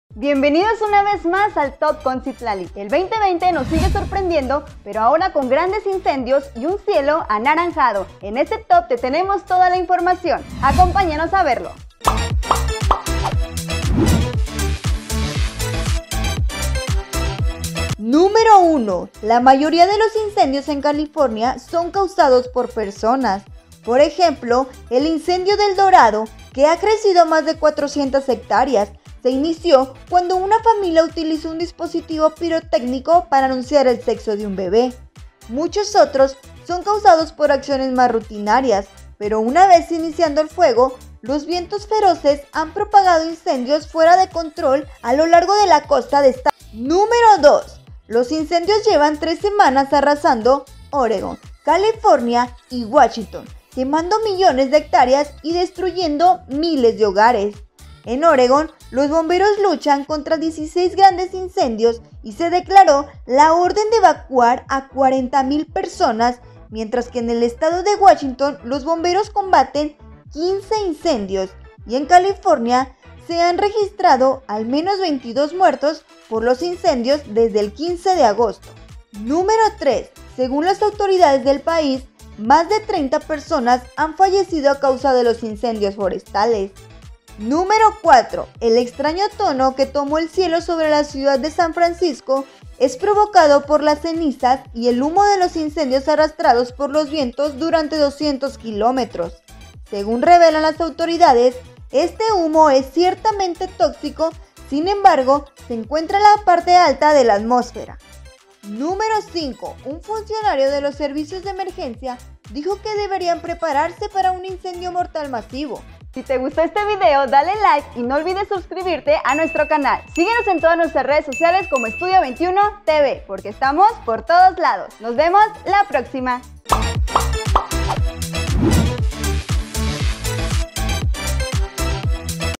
EN LLAMAS; 28 INCENDIOS EN OREGÓN Y CALIFORNIA
ESTADO DE EMERGENCIA; MILES DE AFECTACIONES TRAS LOS INCENDIOS ADEMÁS EL CIELO SE PINTÓ COLOR NARANJA.